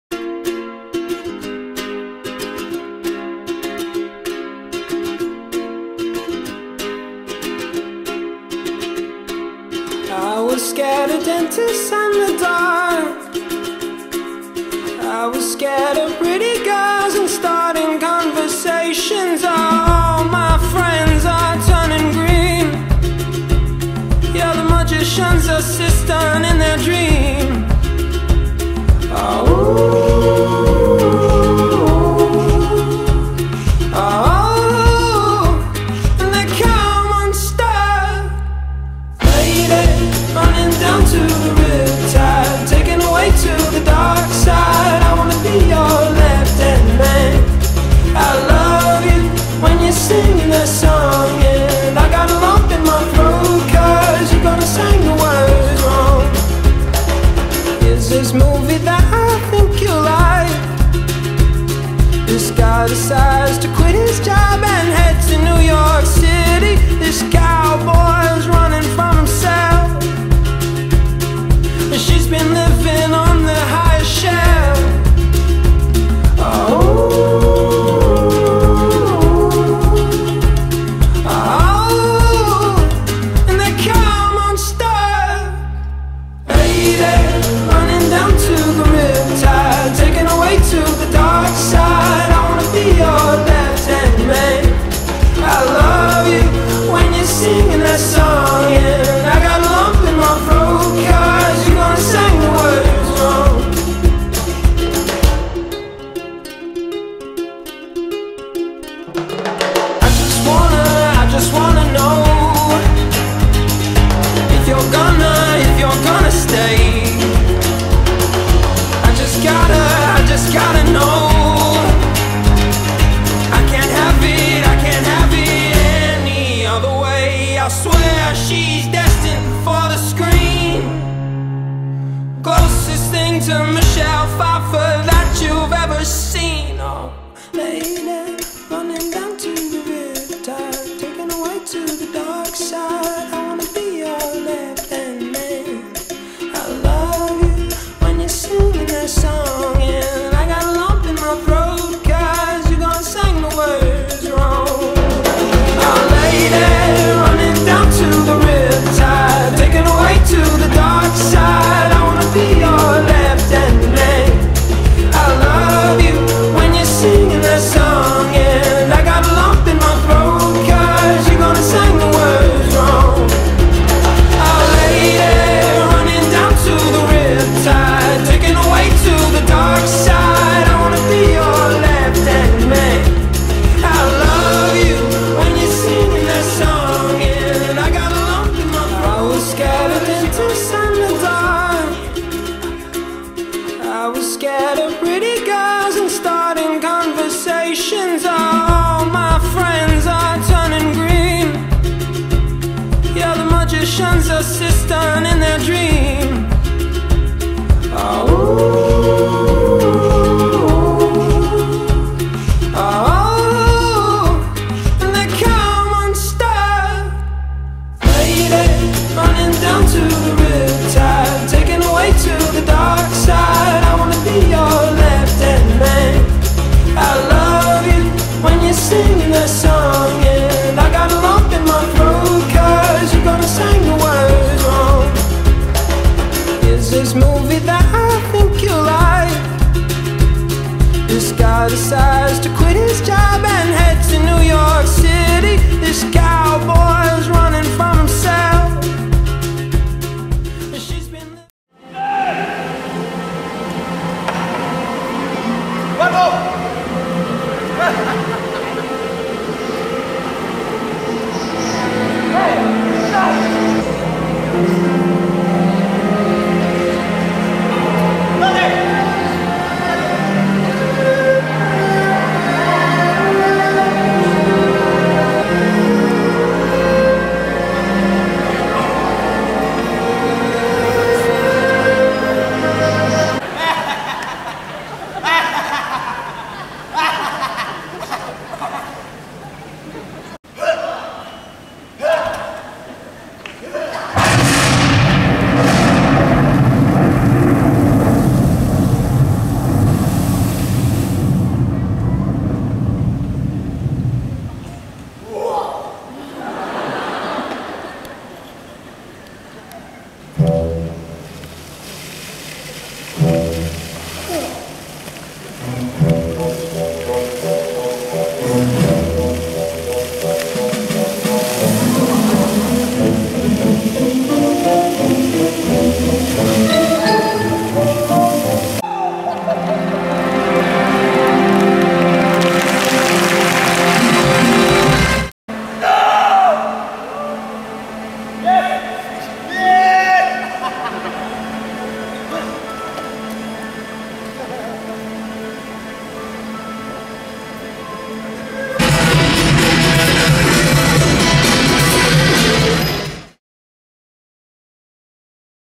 Abdelkader Airour show reel 2019
Abdelkader Airour show reel juin 2019, pbar demo for casting cirque du soleil for the show O